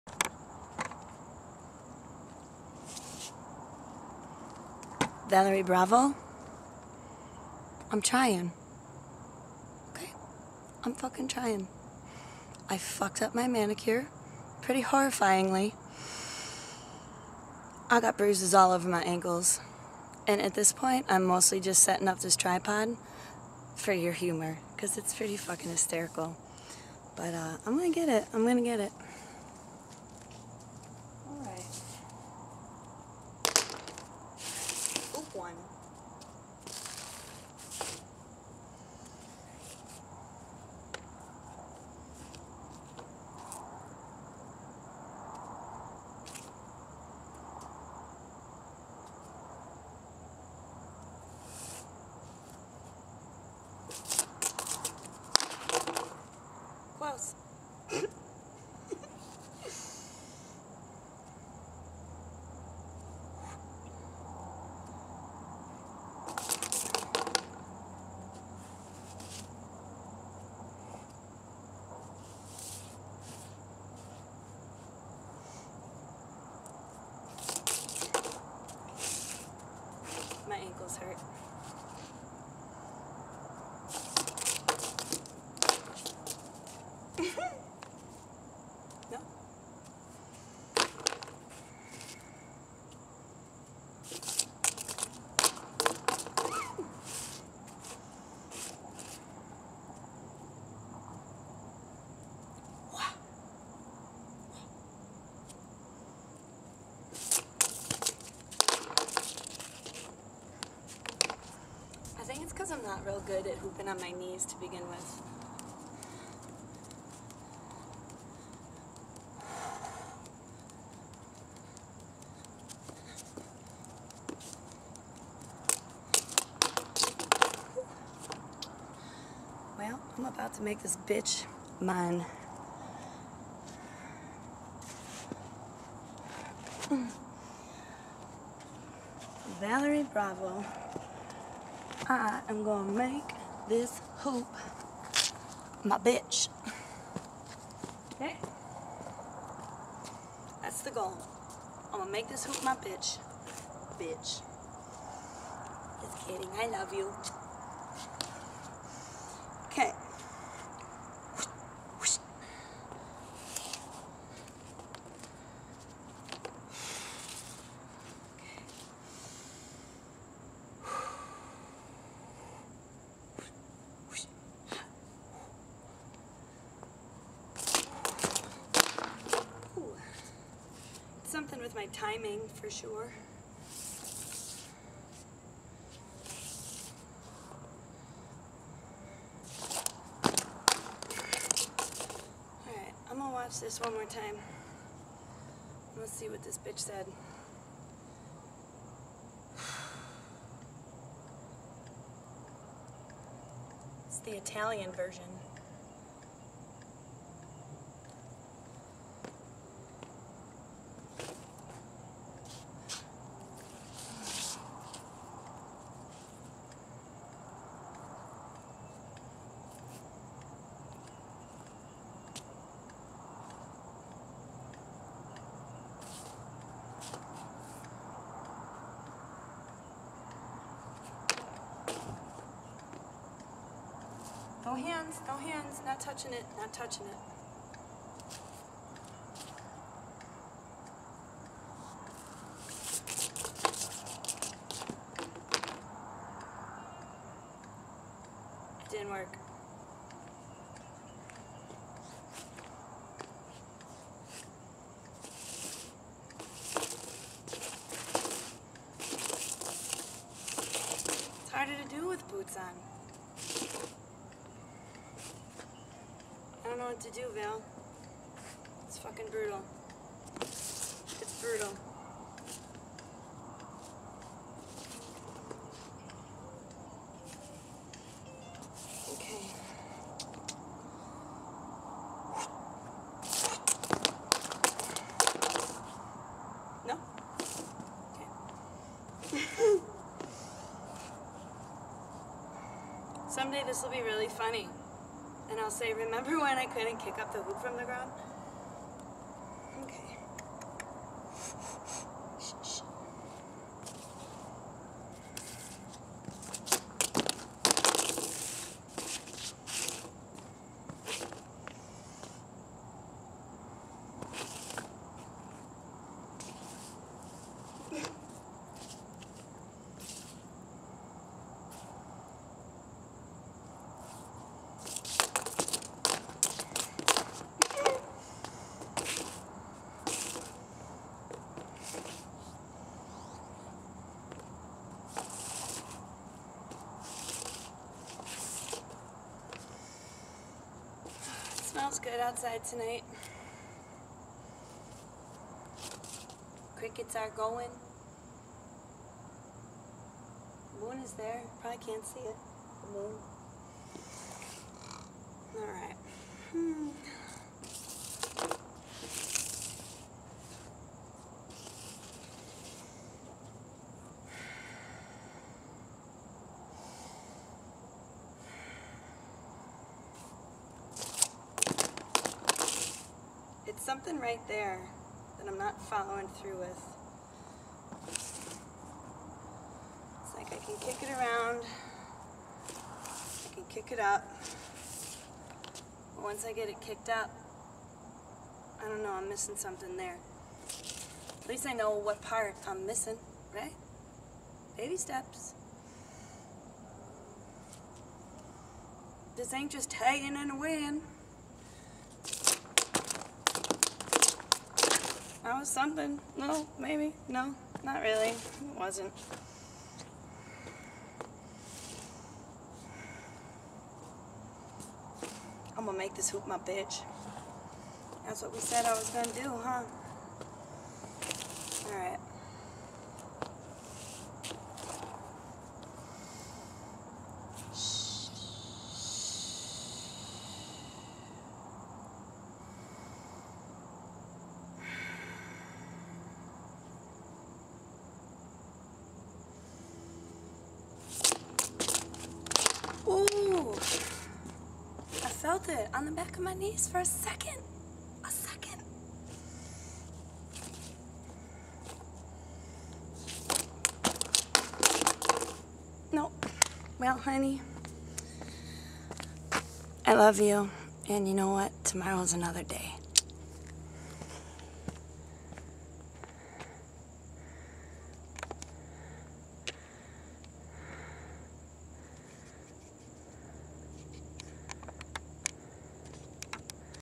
This ain't just Taaeee Innnnnnna Win!
For Valerie Bravo! First attempt at making this hoop my bitch via floor to knee kick up. it's more of a crack up for sure.